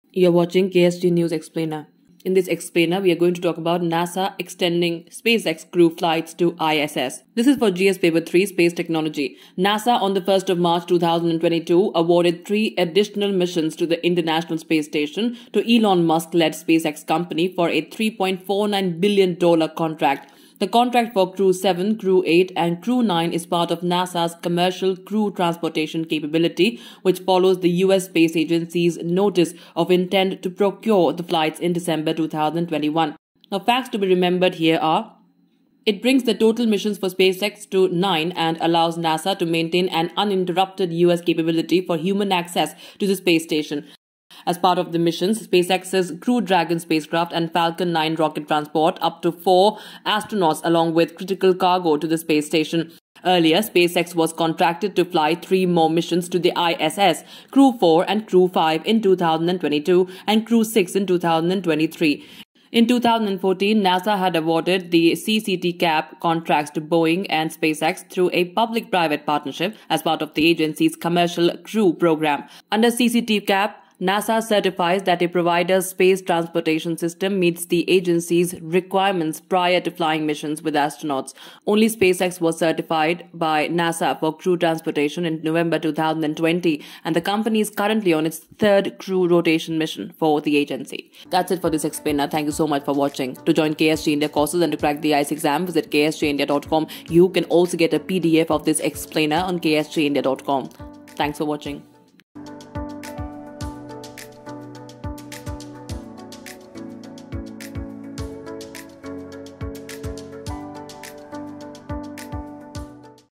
Space Technology : NASA extends SpaceX crew flight contract for 3 missions to ISS
NASA extends SpaceX crew flight to ISS

GS Paper - 3 (Space Technology)

NASA on 1 March 2022 awarded three additional missions to the International Space Station (ISS) to Elon Musk led SpaceX company for a $3.49 billion contract. The contract for Crew-7, Crew-8, and Crew-9 is part of NASA's Commercial Crew Transportation Capability (CCtCap), which follows the US space agency's notice of intent to procure the flights in December 2021.

Topics Covered -
It brings the total missions for SpaceX to nine and allows NASA to maintain an uninterrupted US capability
SpaceX's Crew Dragon spacecraft and Falcon 9 rocket transport up to four astronauts
SpaceX was contracted to fly three more missions
NASA had awarded the CCtCap contracts to Boeing and SpaceX through a public-private partnership
Under CCtCap, NASA certifies that a provider's space transportation system meets the agency's requirements
Only SpaceX was certified by NASA for crew transportation

Time Code -
00:03 - It brings the total missions for SpaceX to nine and allows NASA to maintain an uninterrupted US capability
00:18 - SpaceX's Crew Dragon spacecraft and Falcon 9 rocket transport up to four astronauts
00:45 - SpaceX was contracted to fly three more missions
01:17 - NASA had awarded the CCtCap contracts to Boeing and SpaceX through a public-private partnership
01:25 - Under CCtCap, NASA certifies that a provider's space transportation system meets the agency's requirements
01:38 - Only SpaceX was certified by NASA for crew transportation